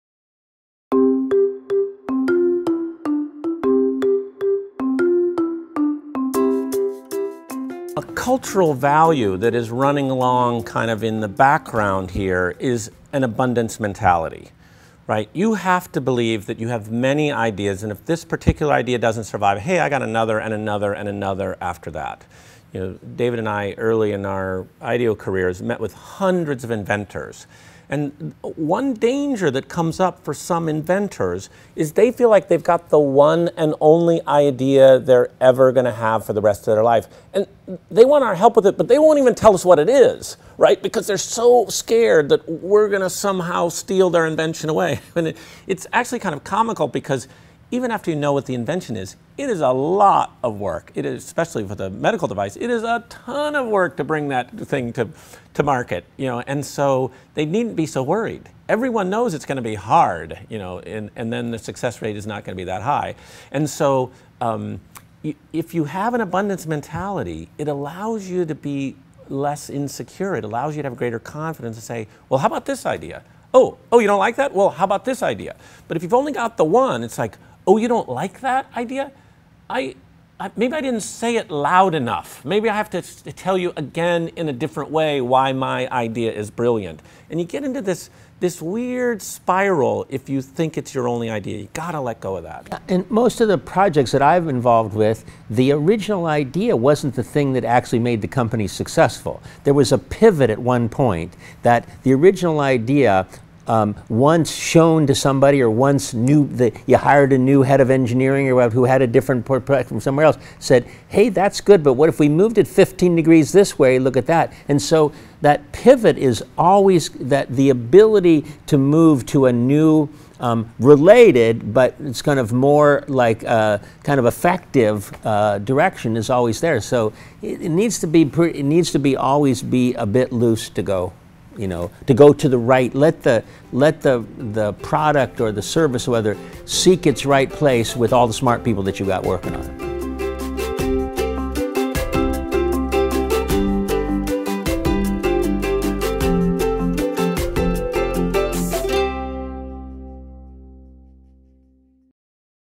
IDEO: On Letting Go of Ideas
David and Tom Kelley of IDEO underscore the importance of not getting too attached to any single idea following ideation.